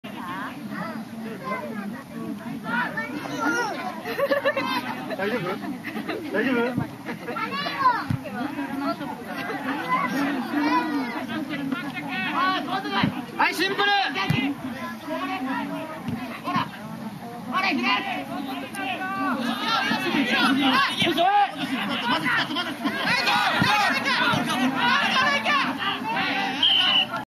Fukushima United vs. Arte Takasaki Practice Match
February 8, 2009. Tohoku Regional League Fukushima United FC vs. JFL side Arte Takasaki. Fukushima would win 2-0. Played in Yaita, Tochigi.